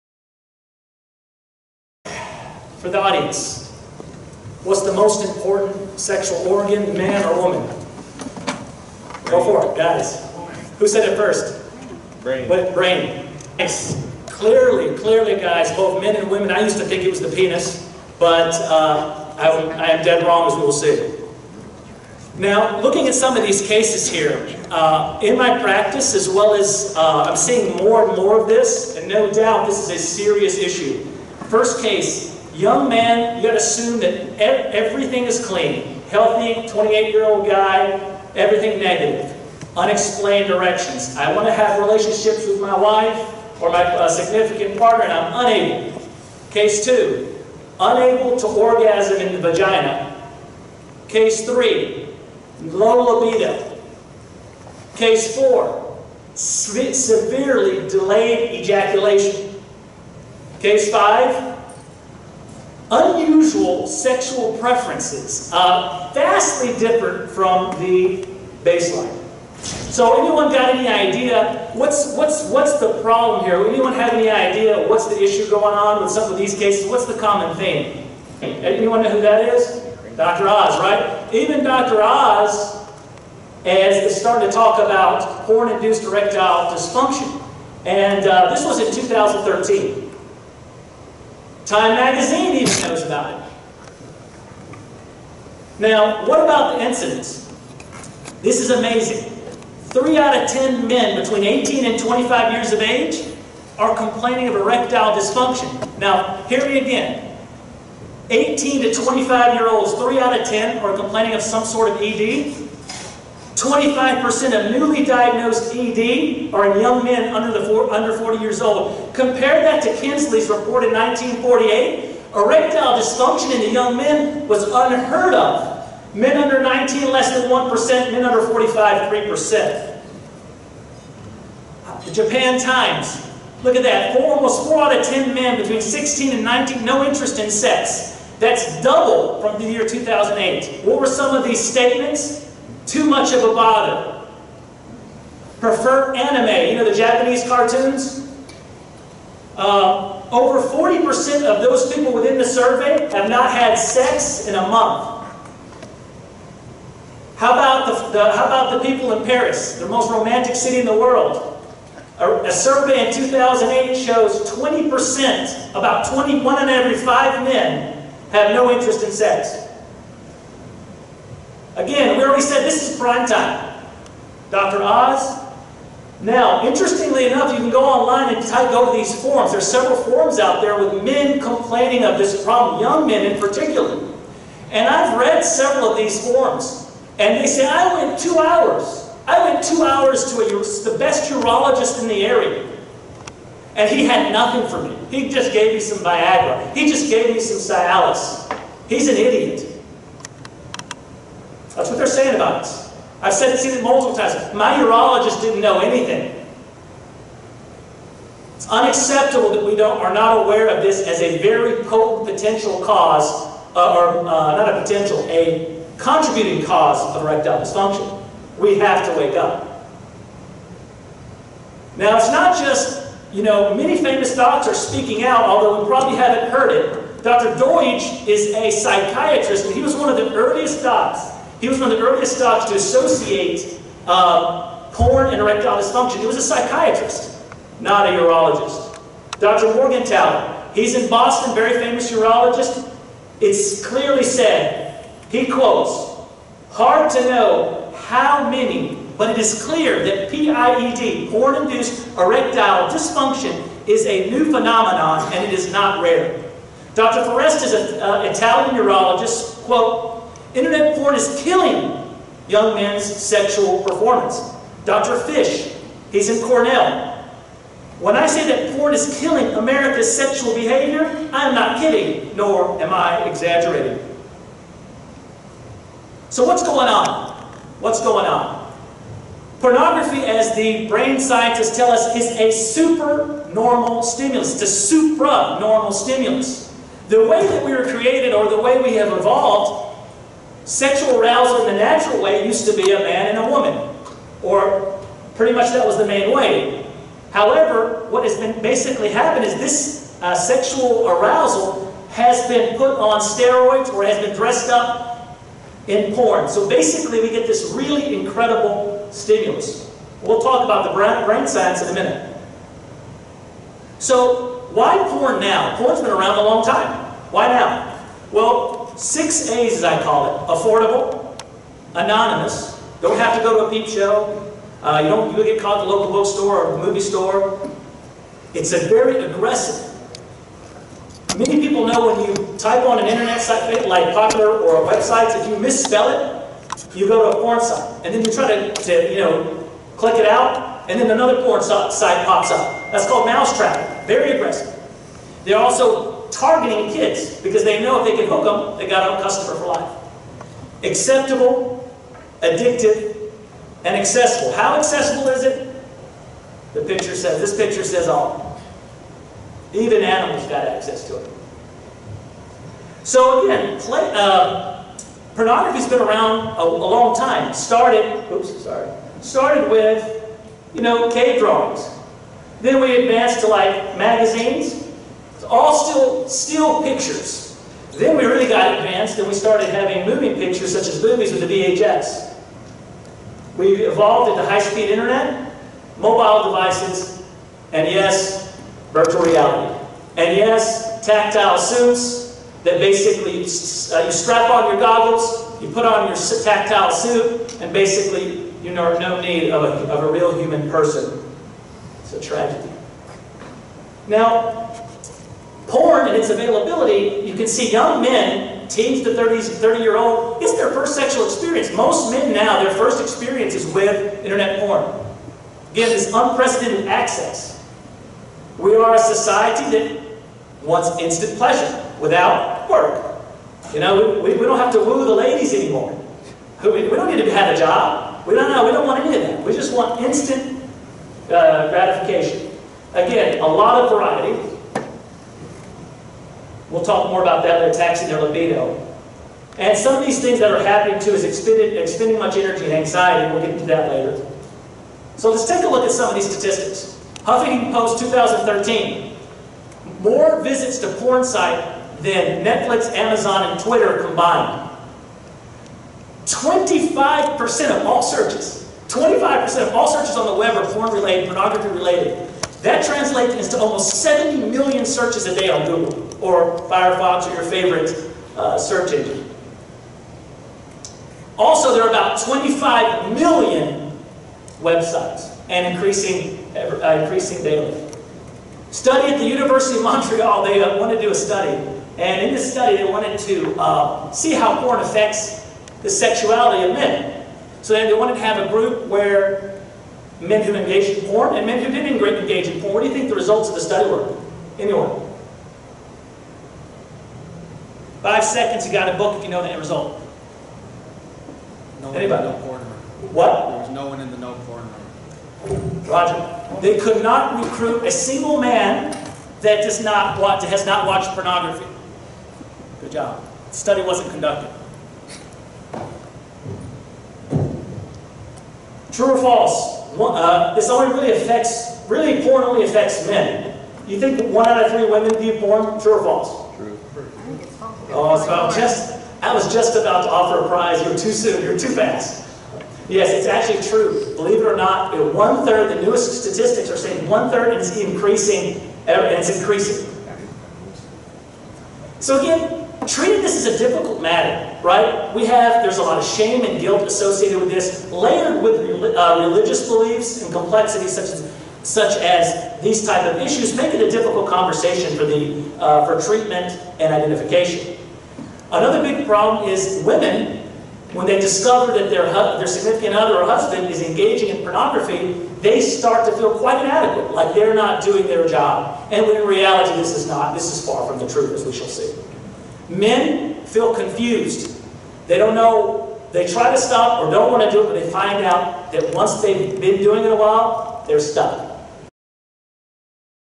Part #1: PORN INDUCED ERECTILE DYSFUNCTION (PIED): problem and scope
Pornography induced erectile dysfunction (PIED): Understanding the scope, science, and treatment. Delivered by Dr. Tarek Pacha at the American Urologic Association in San Diego 2016.